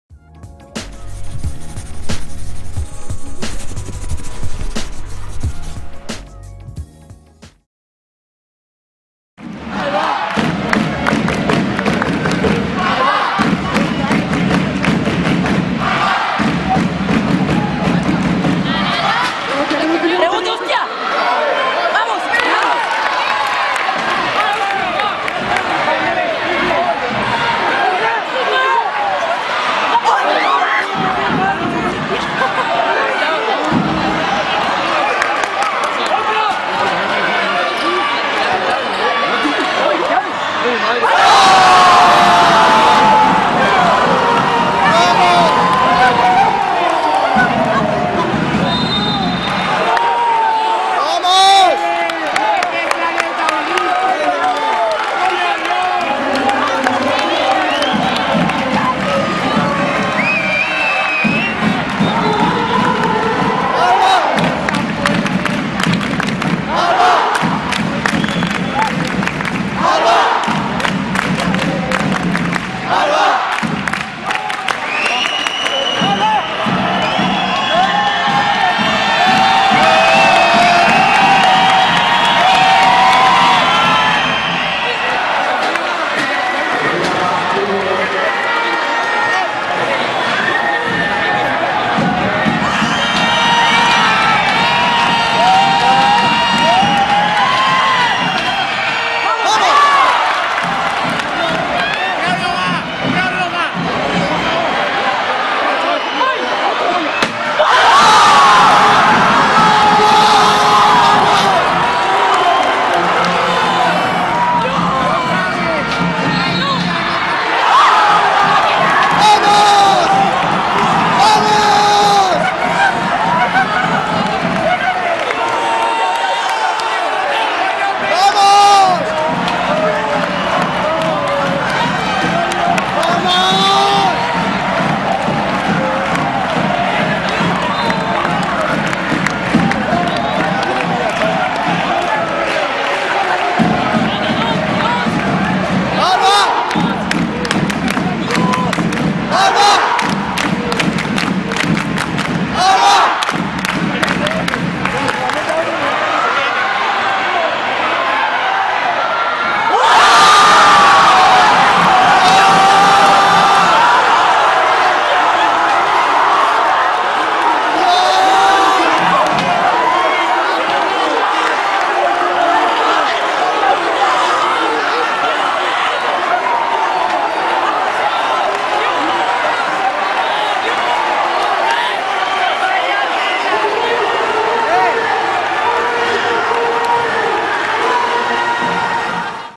APDC | Final histórica - Albacete Basket vs CBV (AUDIO CORREGIDO)
Últimos minutos del partido entre Albacete Basket vs. CBV en el que Garrett Nevels anotó tres triples consecutivos en los segundos finales del partido para dar la victoria y el título de campeón a su equipo.

Imagenes: Albacete Basket